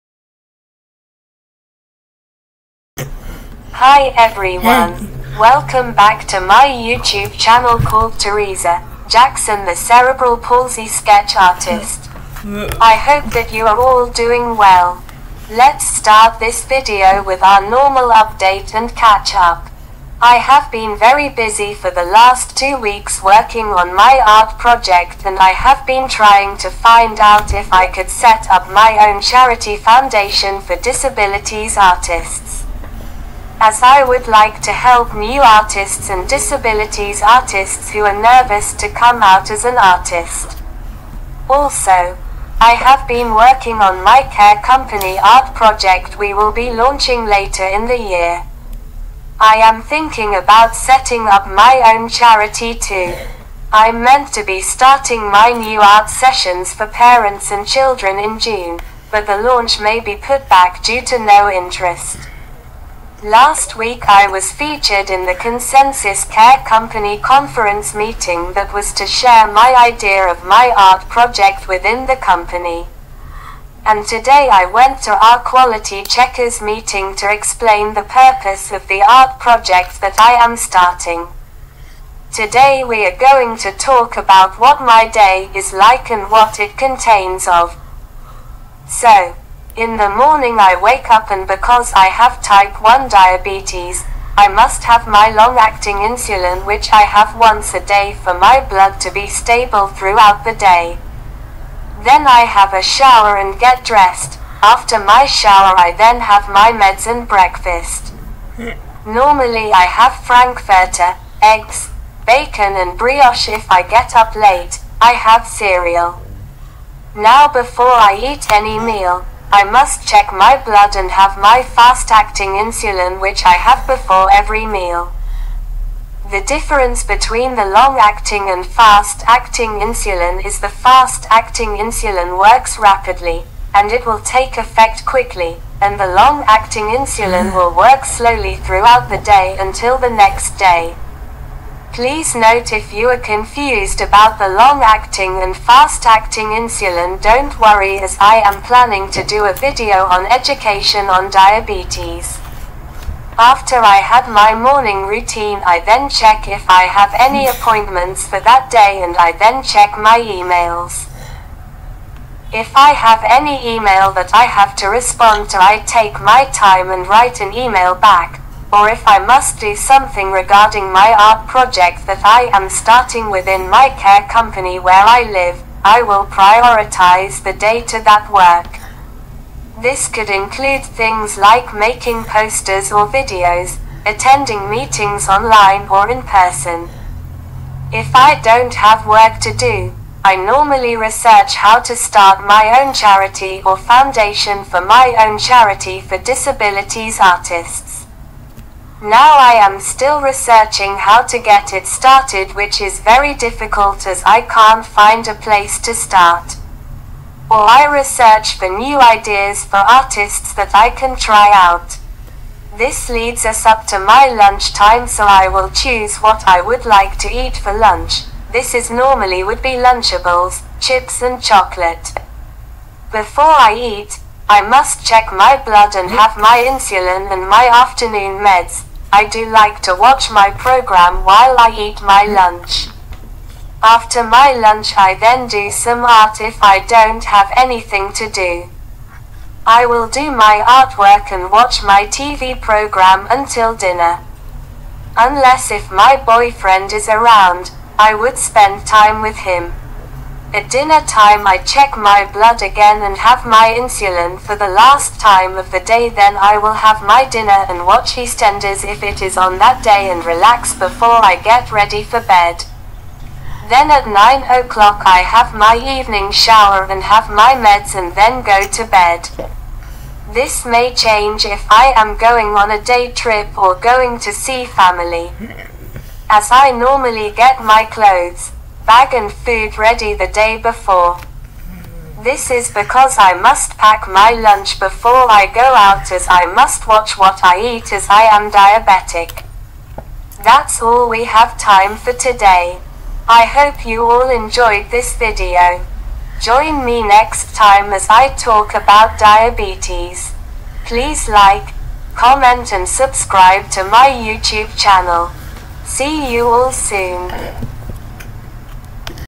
Uncovering My Daily Routine - What I Do to Stay Productive!
in this video I am Talking about how I go about my day on a daily basis and what it contains of for example my diabetes, getting dressed And getting ready for bed.
I even share what a day trip contains too.

I hope that this will help people get a better idea of what a day in a life is like for a artist that has cerebral palsy and and type one diabetes. although I have done a video like this before today I went in depth more about what it Ike for me.